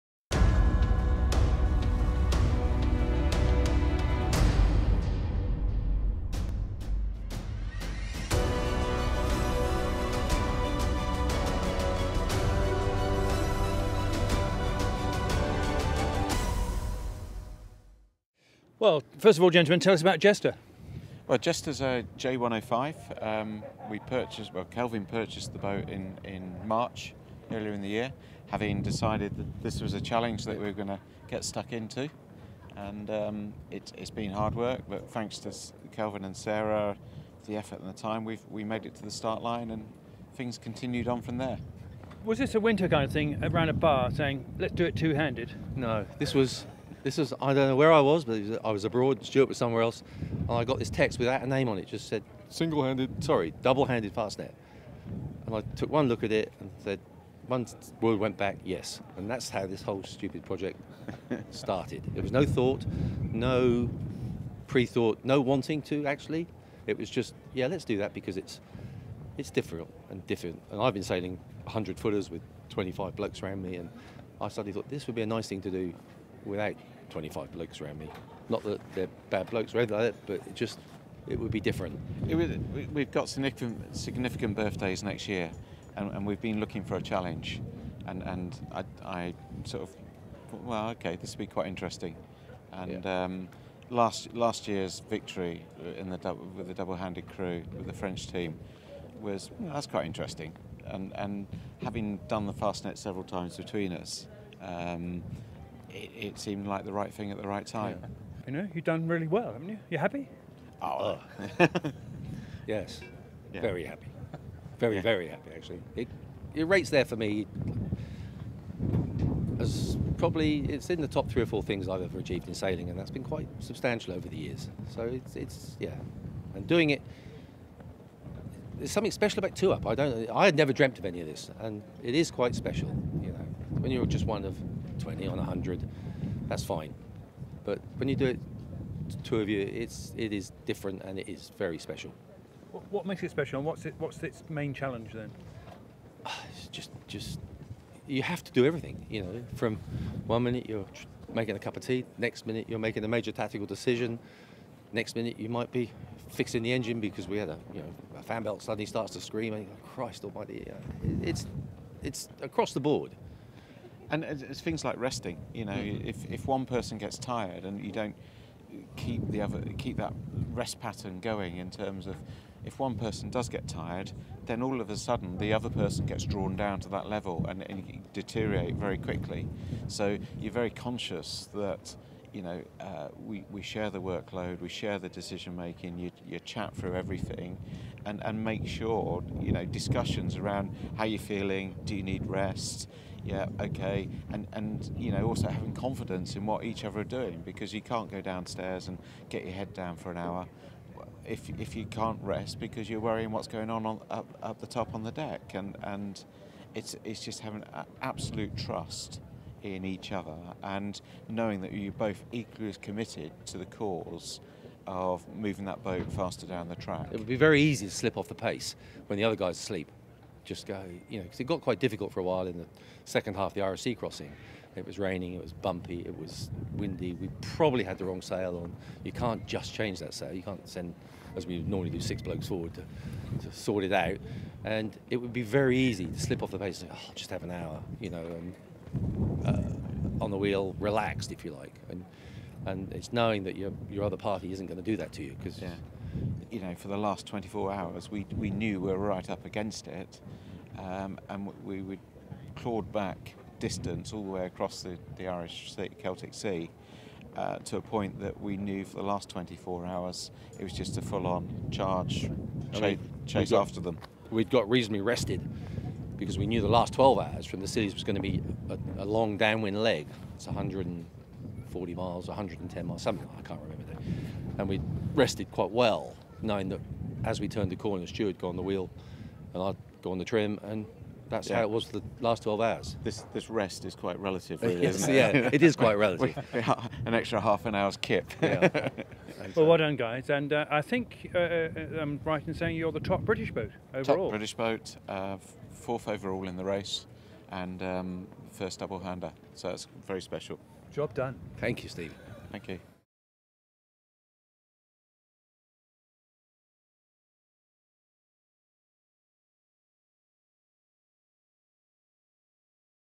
Stuart Childerley and Kelvin Rawlings onJester win the two handed division of the Rolex Fastnet Race
Olympic dinghy sailor Stuart Childerley and long time big boat sailor Kelvin Rawlings have not only won the two-handed class in the Fastnet, but also came 4th in the overall results.  The duo, new to shorthanded sailing, but many Fastnets between them are the first Brits in the Rolex Fastnet Race, that has been dominated by the French this year.